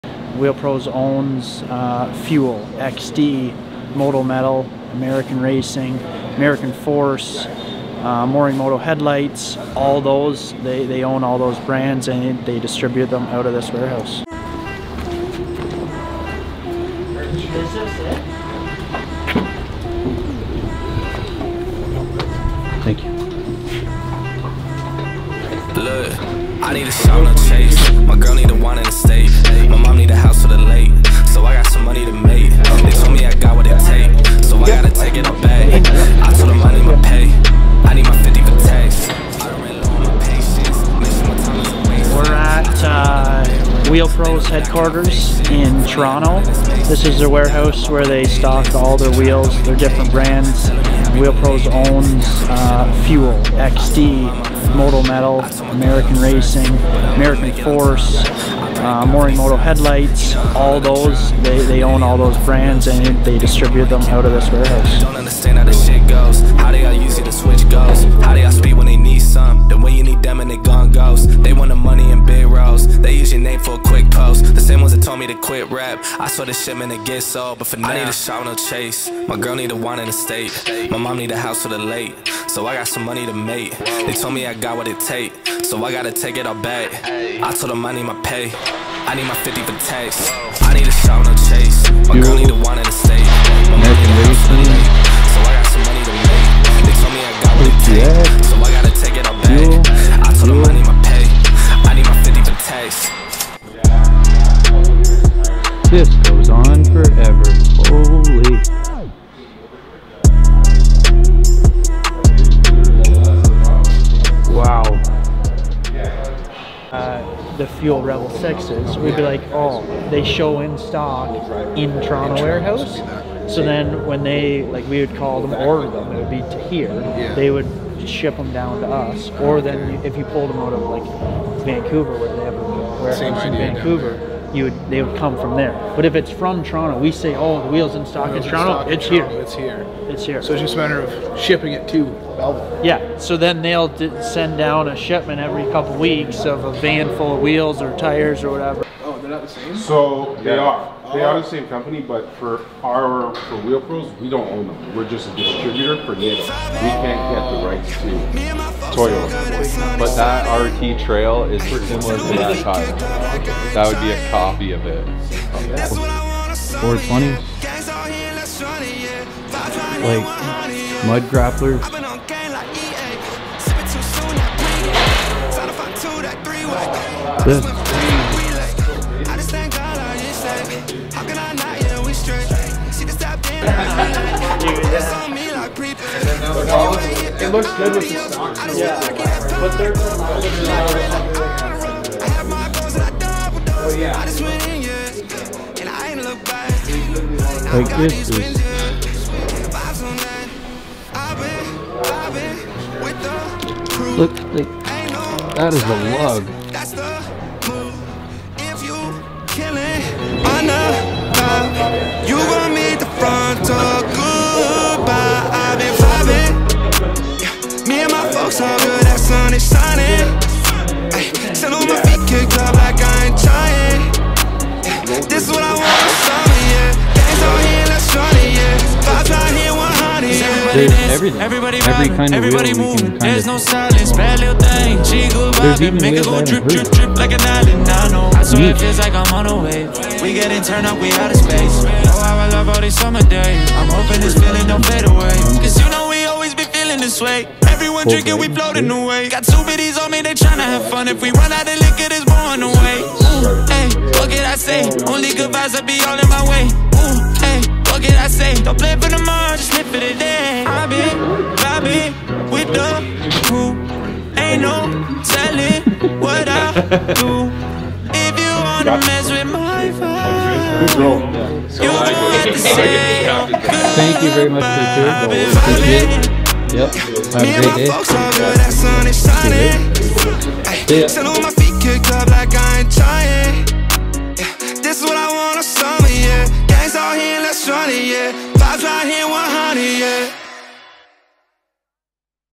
Inside Look at Wheel Pros Headquarters in Toronto, Canada: Tour of Warehouse
Are you curious about what goes on behind the scenes at Wheel Pros, one of the leading wheel and tire companies in North America? In this video, we'll take you on a tour of the Wheel Pros headquarters in Toronto, Canada, where you'll get an up-close look at their state-of-the-art facilities, advanced equipment, and expert team.  From innovative wheel designs to cutting-edge manufacturing techniques, you'll discover what makes Wheel Pros stand out from the competition. Whether you're a car enthusiast or simply interested in the world of wheels and tires, this video is a must-watch!

We are your One Stop 4x4 Specialty Store  We are your Custom and Lifted Truck Specialists!